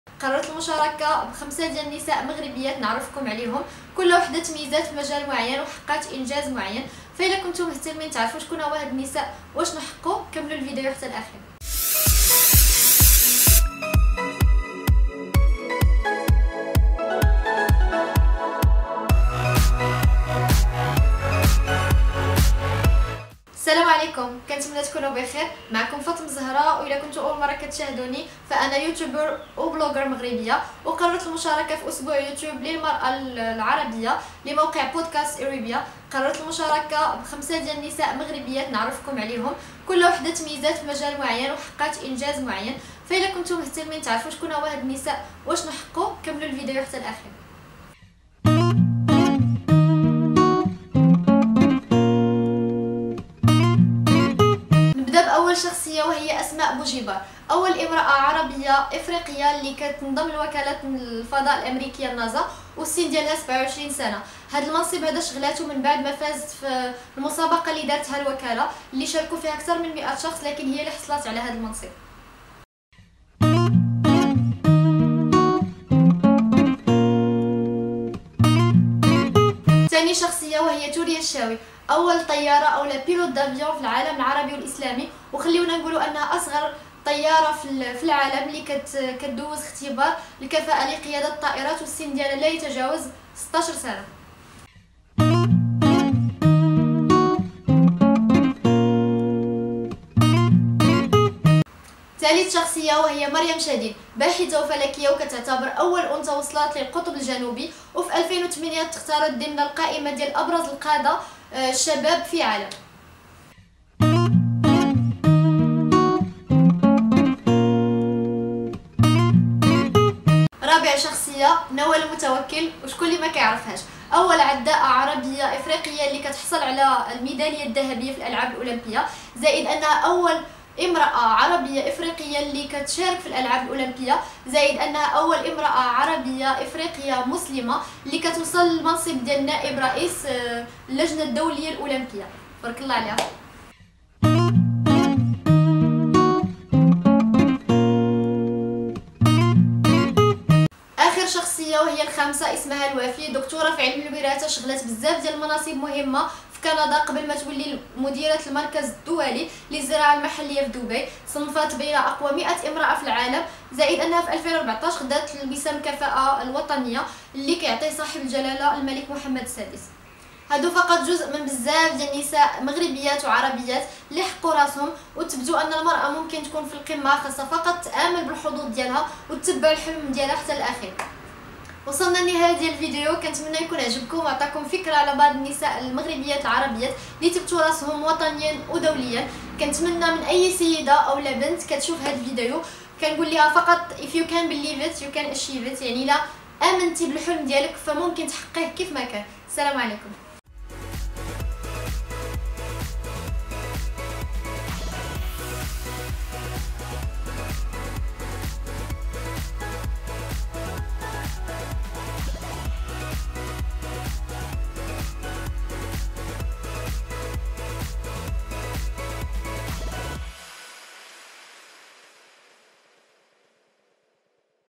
الحقيقة التي لا تعرفها عن بنات المغرب 😮 In line with Tima
هل المرأة المغربية حقا كما يقال عنها؟ أم أنها امراة ناجحة حققت انجازات كثيرة وتفوقت حتى على الرجال؟ هل هناك امرأة عربية تشتغل في وكالة الفضاء الأمريكية ناسا؟ نعم هي أسماء بوجيبار. من هي أول طيارة عربية؟ هي المغربية ثريا الشاوي. من هي أول امراة تصل إلى القطب الجنوبي؟ هي المغربية مريم شديد. من هي أول عداءة عربية تتحصل على ميدالية في الألعاب الأولمبية؟ هي المغربية نوال المتوكل. من هي المرأة العربية التي تألقت في كندا؟ هي المغربية اسمهان الوافي. كل هذه الأسئلة وأسئلة أخرى تجيب عنها فاطمة الشافي في فيديو مخصص للمشاركة في الدورة الثالثة من مسابقة أسبوع يوتيوب المرأة العربية التي تنظمها بودكاست آرابيا.